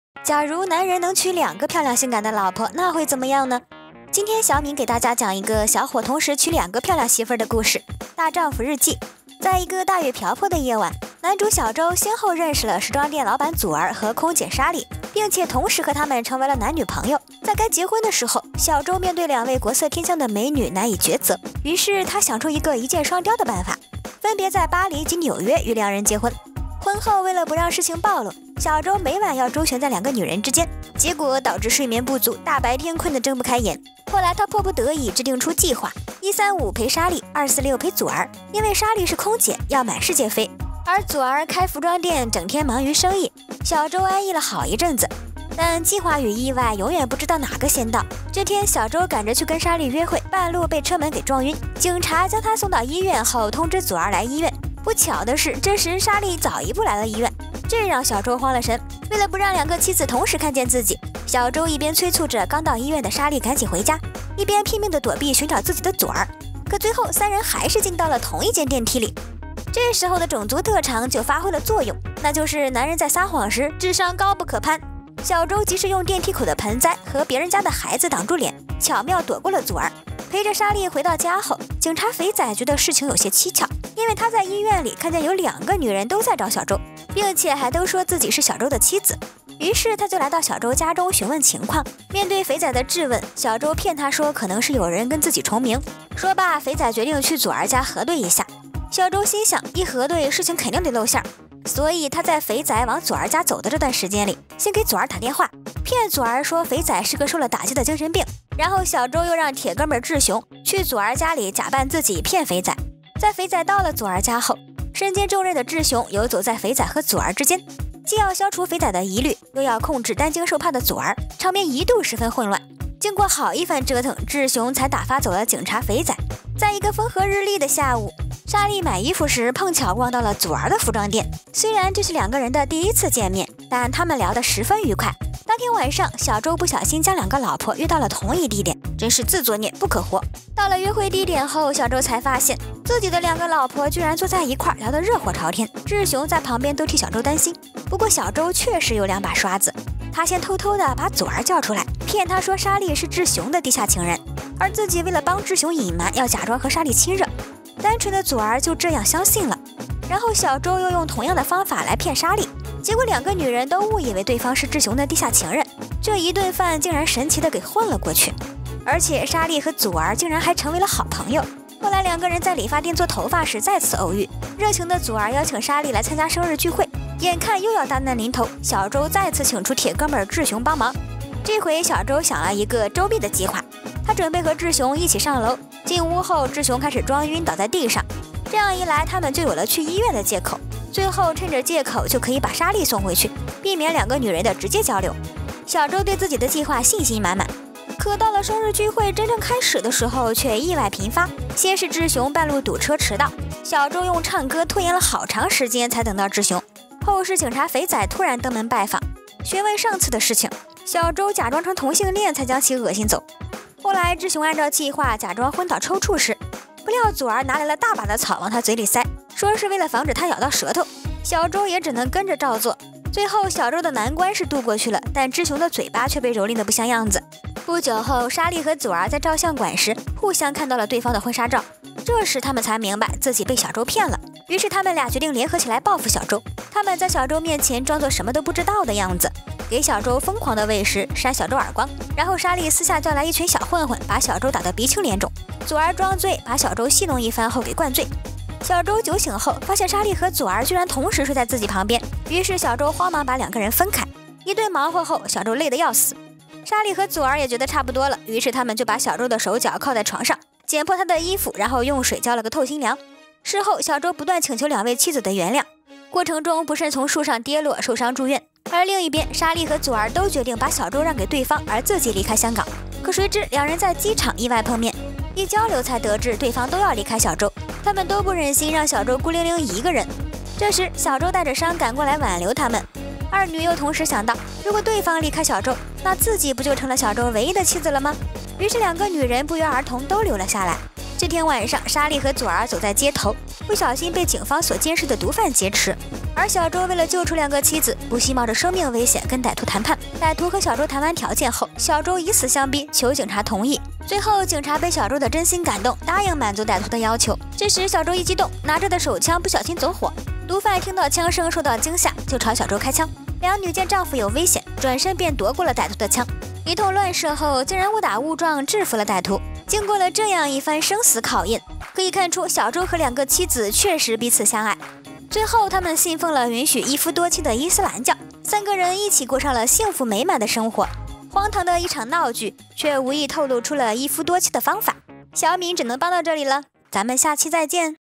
男子一次娶两个老婆，双倍工作量让他筋疲力尽，痛并快乐着
影片讲述金融商人周定发恋上时装店老板祖儿、空中小姐莎莉，难以抉择，最后分别在巴黎及纽约与两人结婚。为免两人得知，周利用小聪明屡次化险为夷，周旋于两个老婆之间。

大家好！这里是晓敏讲故事的YouTube官方频道

晓敏讲故事，做最走心电影解说者！